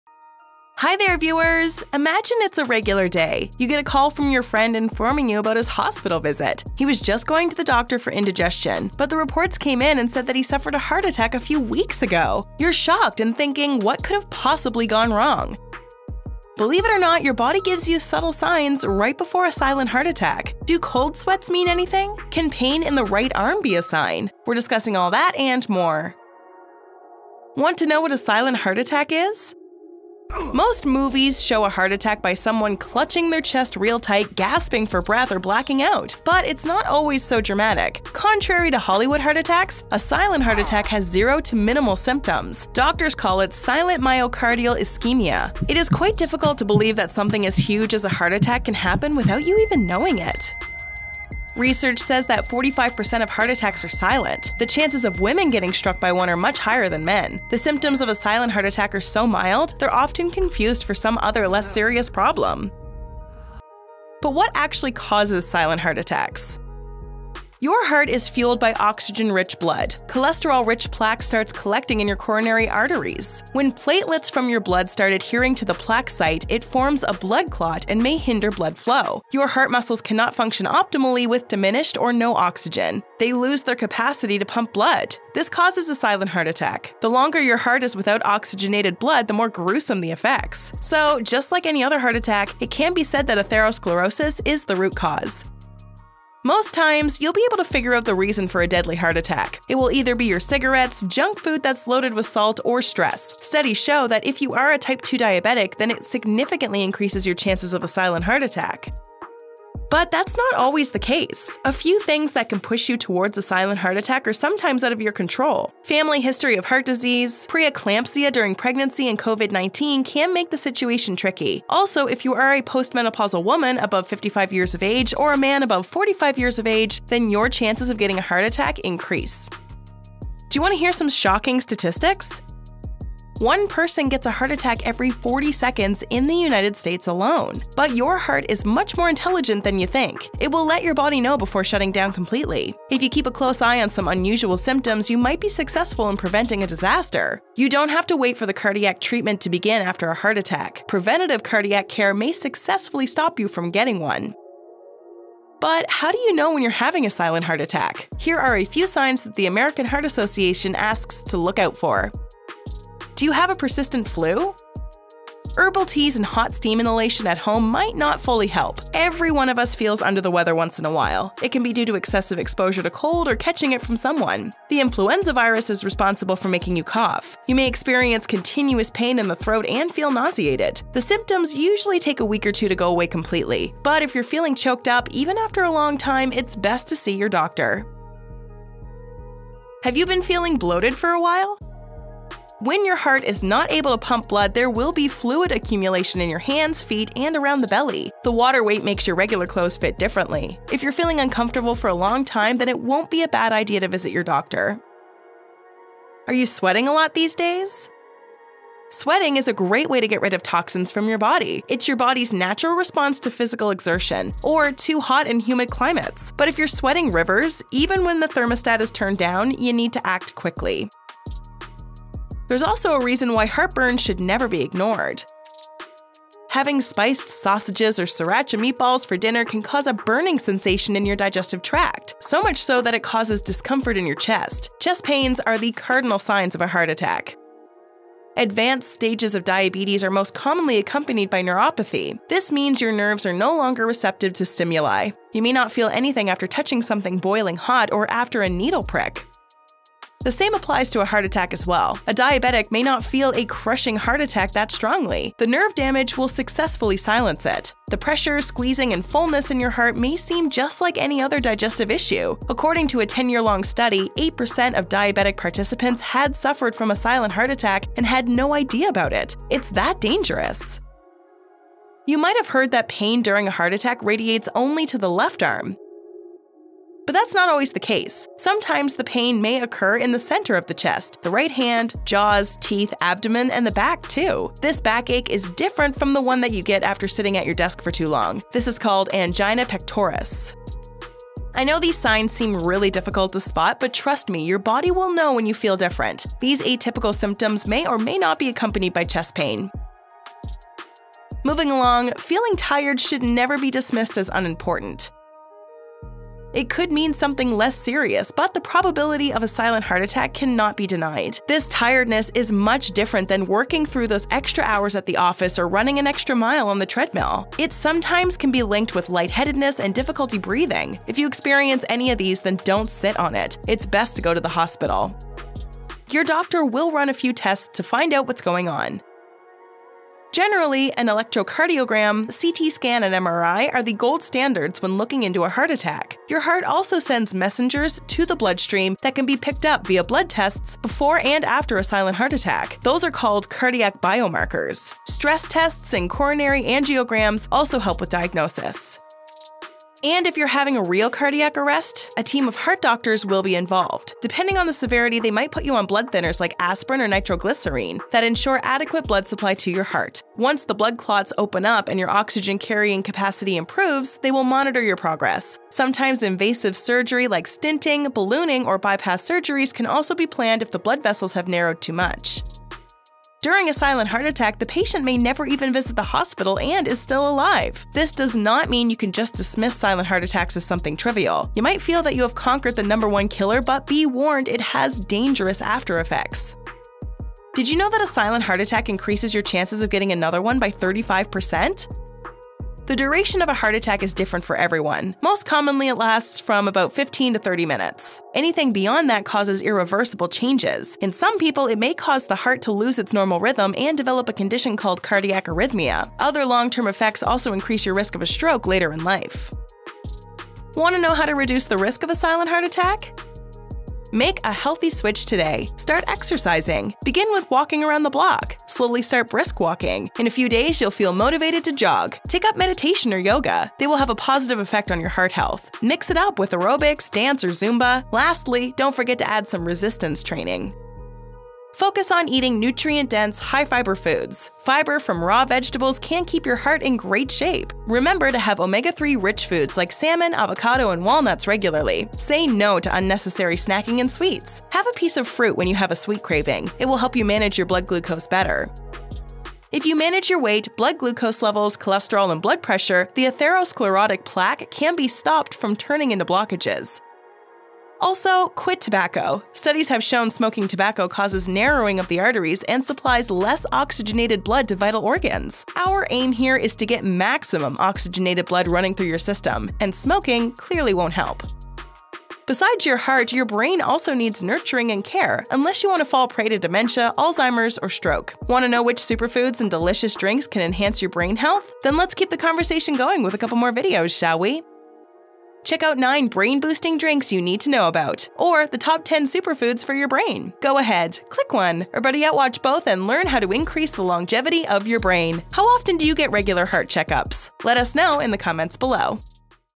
6 Signs Of A Silent Heart Attack That Are Always Ignored
Imagine it's a regular day. You get a call from your friend informing you about his hospital visit. He was just going to visit the doctor for indigestion. But the reports came in saying that he suffered a heart attack a few weeks ago. You’re shocked and thinking, what could have possibly gone wrong?

Believe it or not, your body gives you subtle signs right before a silent heart attack. Do cold sweats mean anything? Can pain in the right arm be a sign? We are discussing all that and more.

Using garlic extract can help your heart health. Here's the link to buy Extra Strength Garlic Extract

Timestamps:
Intro - 0:00
Want to know what a silent heart attack is? - 00:27
But what actually causes silent heart attacks? - 01:06
Do you want to hear some shocking statistics? - 02:12
But how do you know when you're having a silent heart attack? Here are a few signs that the American Heart Association asks to look out for.  - 02:37
Have you been feeling bloated for a while? - 03:10
Are you sweating a lot these days? - 03:27
There is also a reason why heartburn should never be ignored. - 03:45
You might have heard that pain during a heart attack radiates only to the left arm. - 04:33
Moving along, feeling tired should never be dismissed as unimportant. - 05:04
Your doctor will run a few tests to find out what's going on. - 05:27
Did you know that a silent heart attack increases your chances of getting another one by 35%? - 06:27
Want to know how to reduce the risk of a silent heart attack? - 06:51

Summary:
Want to know what a silent heart attack is?
Most movies show a heart attack by someone clutching their chest real tight, gasping for breath, or blacking out.

But what actually causes silent heart attacks?
Your heart is fueled by oxygen-rich blood. Cholesterol-rich plaque starts collecting in your coronary arteries.

Do you want to hear some shocking statistics?
One person gets a heart attack every 40 seconds in the United States alone. But your heart is much more intelligent than you think.

Do you have persistent flu?
Herbal teas and hot steam inhalation at home might not fully help. Every one of us feels under the weather once in a while.

Have you been feeling bloated for a while?
When your heart is not able to pump enough blood there will be fluid accumulation in your hands, feet, and around the belly.

Are you sweating a lot these days?
Sweating is a great way to get rid of the toxins from your body. It's your body's natural response to physical exertion or too hot and humid climates.

There is also a reason why heartburn should never be ignored.
Having spiced sausages or sriracha meatballs for dinner can cause a burning sensation in your digestive tract. So much so that it causes discomfort in your chest.

You might have heard that pain during a heart attack radiates only to the left arm.
But that's not always the case. Sometimes the pain may occur in the center of chest, the right hand, jaws, teeth, abdomen, and the back, too.

Moving along, feeling tired should never be dismissed as unimportant.
It could mean something less serious but the probability of a silent heart attack cannot be denied. This tiredness is much different than working through those extra hours at the office or running an extra mile on the treadmill.